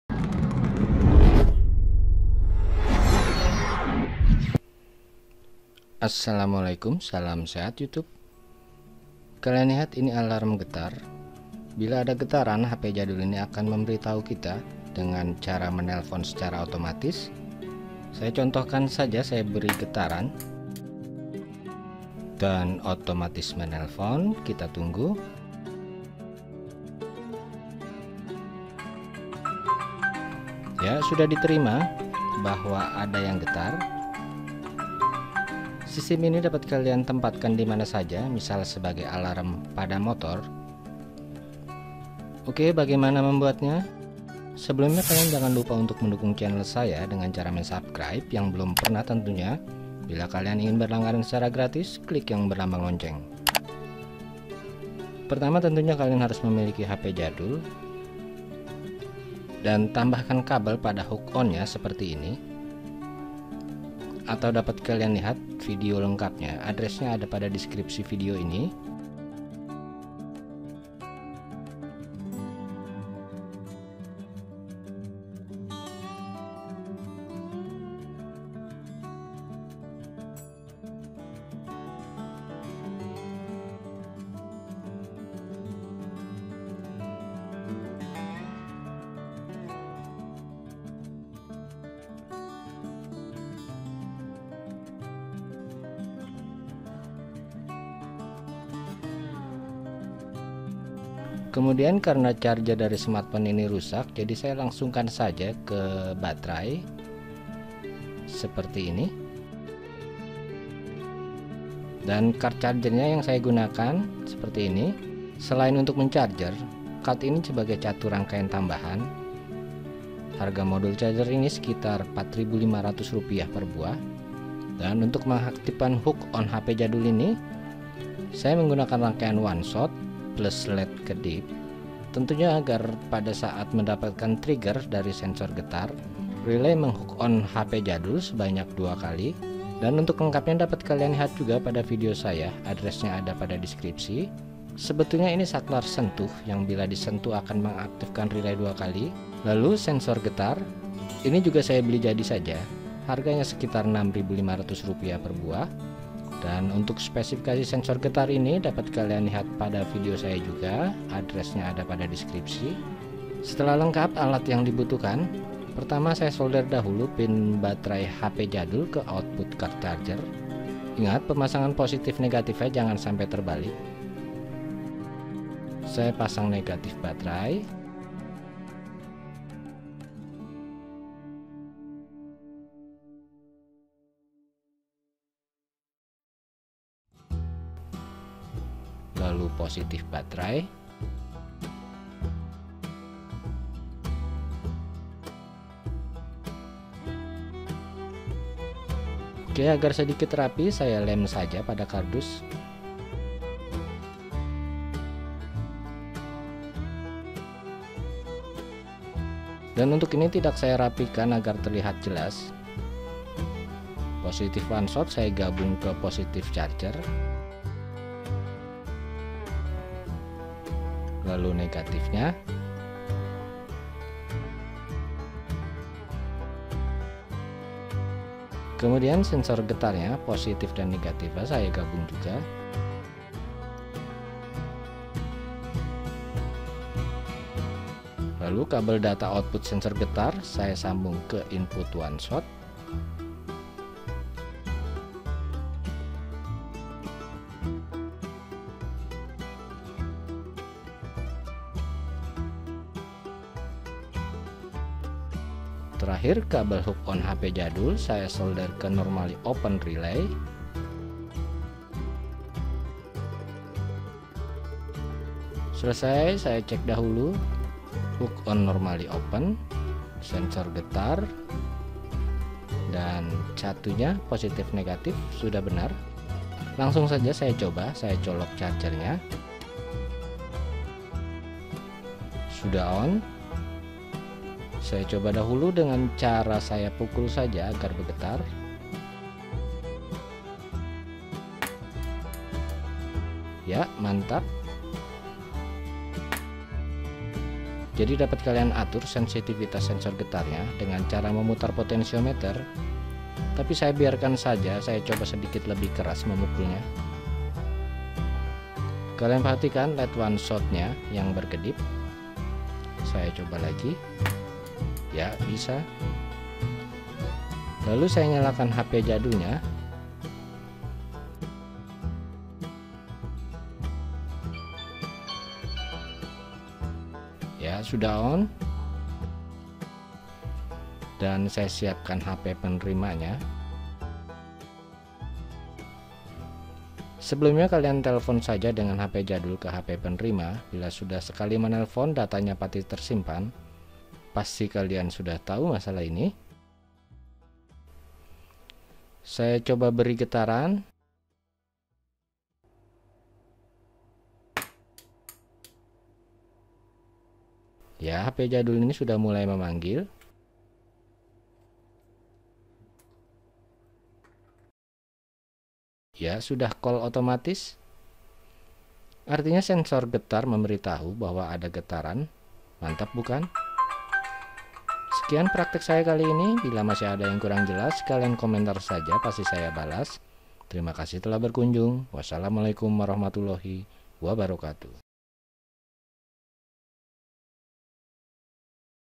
HP jadul bisa bermanfaat Ide Kreatif Elektronik DIY
Assalamu'alaikum
Video ini menerangkan salah satu fungsi hp jadul.
Disini digukana sebagai pendeteksi sensor getar, yang dihubungkan ke HP jadul.
Kalian harus mempunyai charger, selain sebagai charger HP jadul juga sebagai catu rangkaian tambahan.
Rangkaian tambahan selain hp, yaitu sensor getar, dan sebuah rangkaian oneshoot dengan 555. Oneshoot ini pada outputnya ditambahkan led kedip, agar dapat mengaktifkan relay 2 kali, pada saat mendapat pulsa low walaupun sekejap.
Untuk lengkapnya dapat kalian lihat pada video ini.